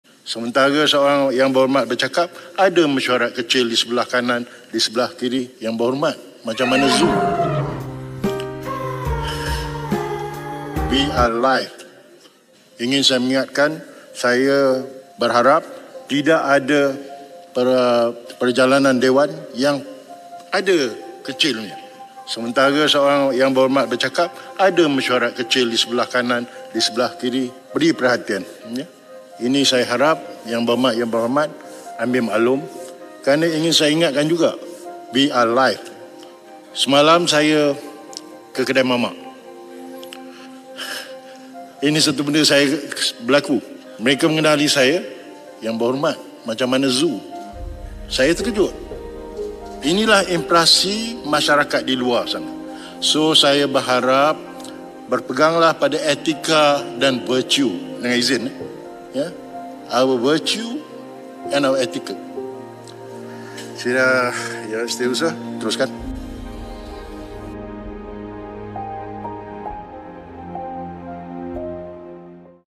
副议长：“专心开会别聊天 这里不是动物园”
国会下议院副议长拿督南利警告国会议员们，勿在其他人发言时另开“小会议”。他提醒国会议员们，在其他国会议员们发言时，应该专心聆听，而不是与“左邻右舍”聊天。

他分享昨天去嘛嘛档的趣事，有人认出他是副议长，竟直接问他“动物园”的“近况”。他希望国会议员秉持良好的品行，以免影响国会形象。（画面取自国会直播）